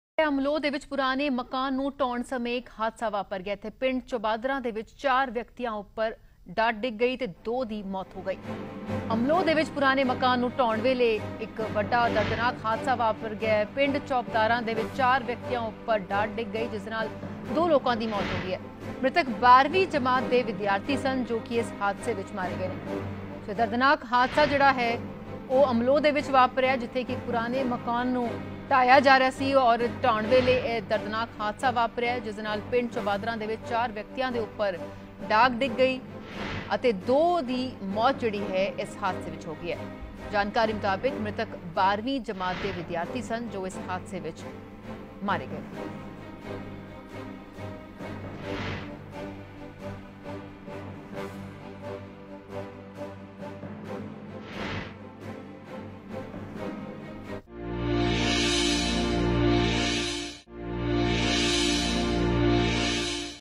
Amloh 'ਚ ਪੁਰਾਣੇ ਮਕਾਨ ਨੂੰ ਢਾਹੁਣ ਸਮੇਂ ਵਾਪਰਿਆ ਹਾਦਸਾ
🔹4 ਵਿਅਕਤੀਆਂ ਉਪਰ ਡਿੱਗੀ ਛੱਤ, 2 ਦੀ ਹੋਈ ਮੌਤ
🔹ਮ੍ਰਿਤਕ 12ਵੀਂ ਜਮਾਤ ਦੇ ਸਨ ਵਿਦਿਆਰਥੀ